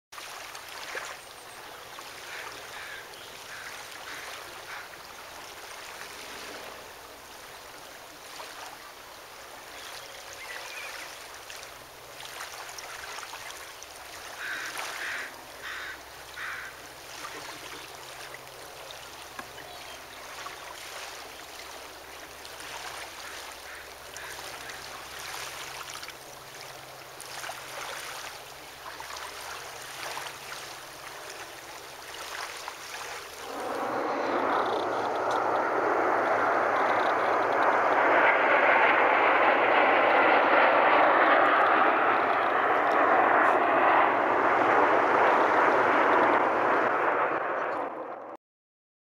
Boeing 737 WestJet Coming in to Comox airport
Boeing 737 WestJet Coming in for landing at Comox airport on Vancouver Island Canada